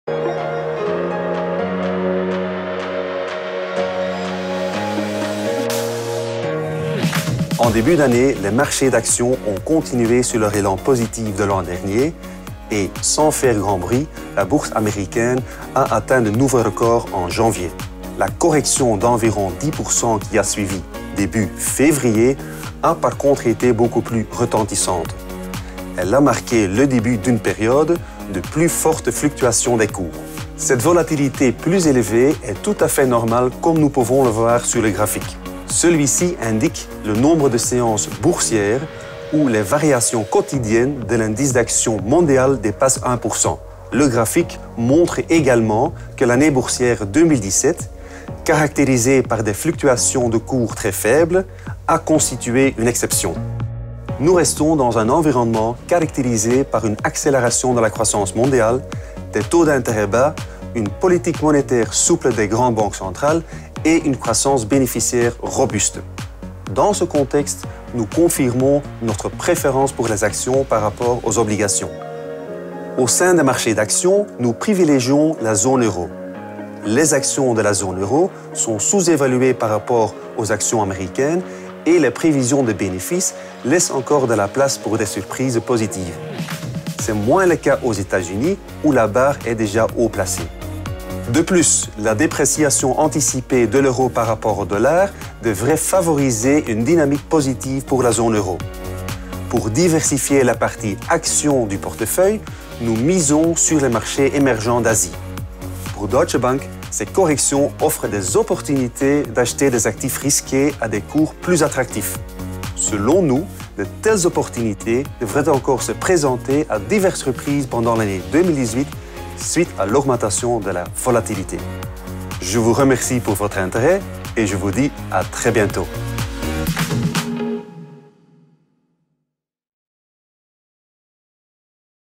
| DB Market Update | Fin de la tendance haussière ou opportunité d’achat ?
Après avoir démarré le mois de janvier en trombe, les bourses mondiales ont dû faire face à des turbulences. Ce regain de volatilité était attendu, mais de nombreux investisseurs n’étaient plus habitués à de telles conditions de marché, étant donné la période exceptionnellement calme observée en 2017. S'agit-il dès lors d'une correction temporaire ou d'un retournement de tendance ?